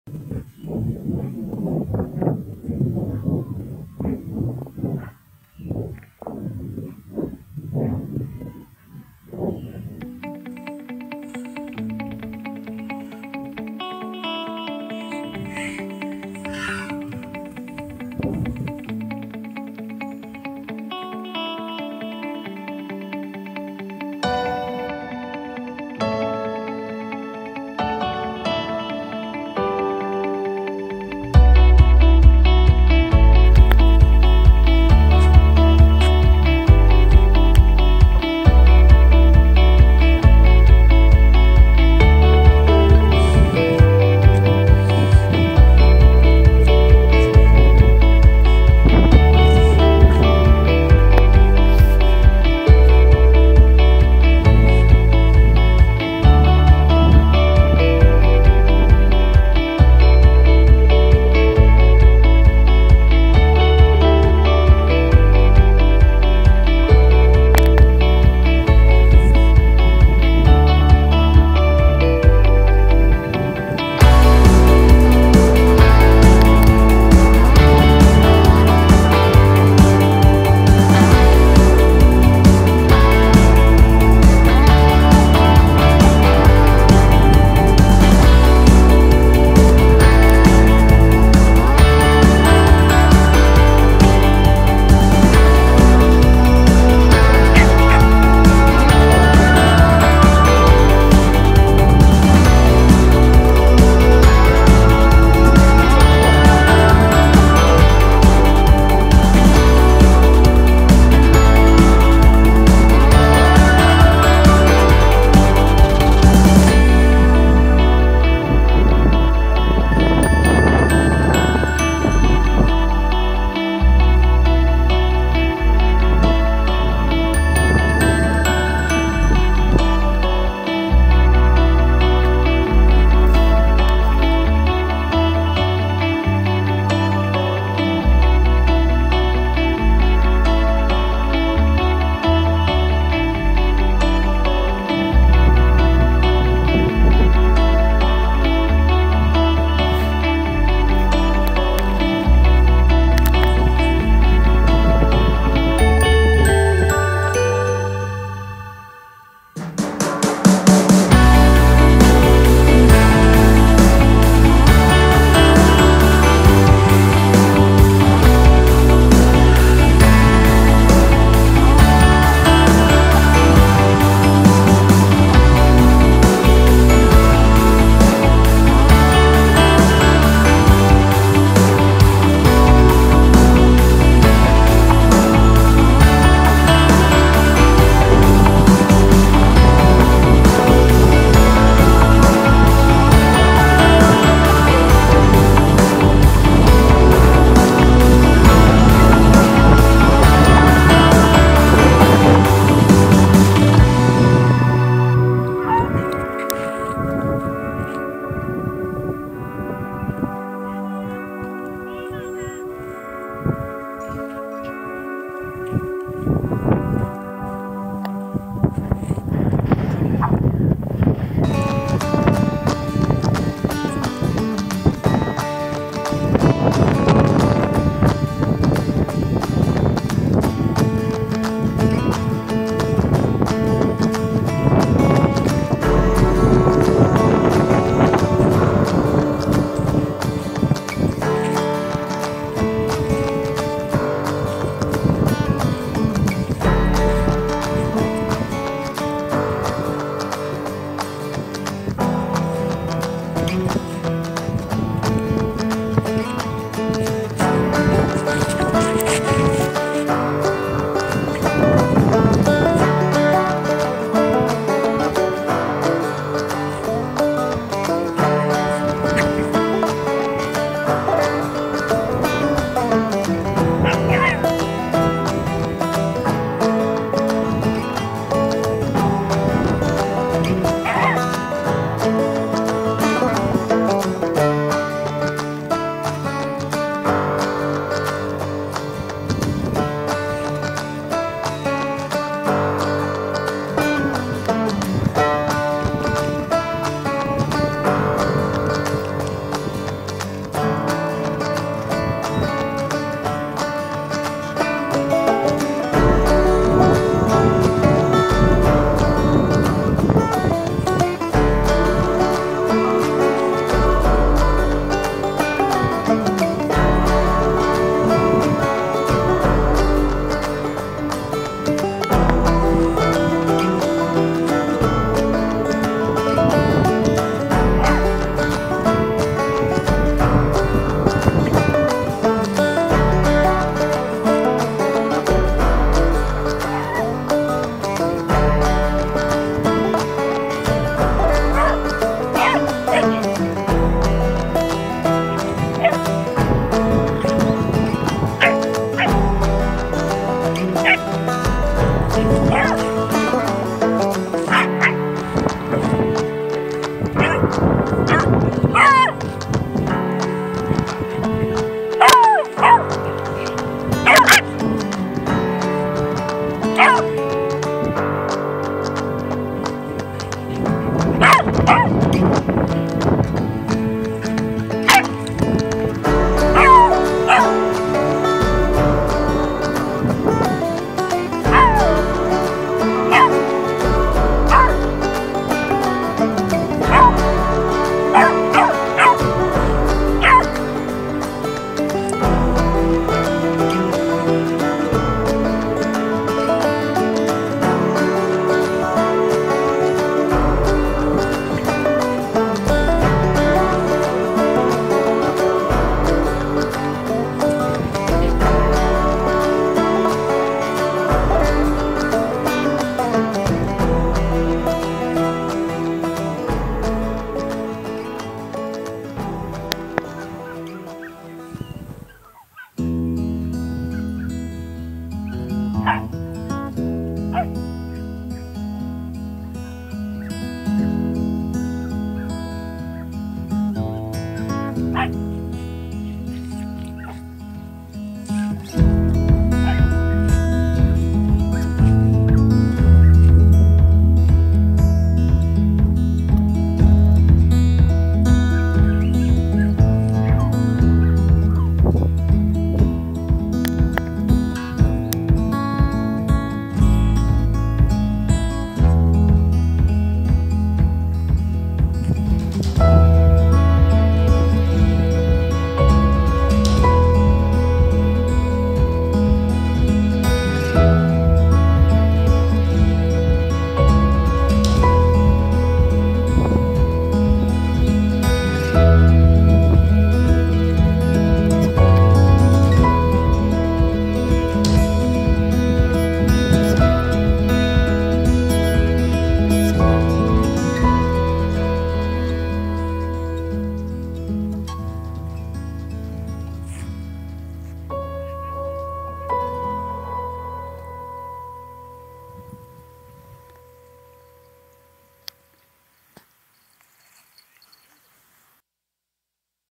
堺市★うみふれのドッグラン♪
こまつと堺市にあるドッグランへ行ってきました。ここは、アルもモナも来たことがあります。モナは前回のランで疲労が出たので家で休ませています。